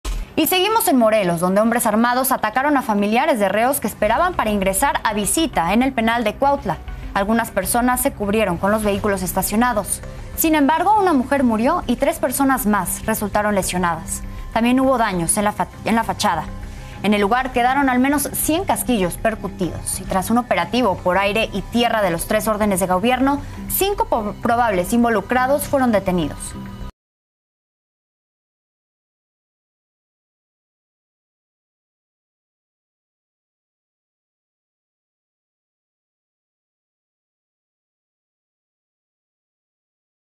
Hombres armados atacan a familiares de reos en Penal de Cuautla - Las Noticias
Hombres armados atacaron a familiares de reos que esperaban para ingresar a la visita en el penal de Cuautla; algunas personas se cubrieron con los vehículos estacionados.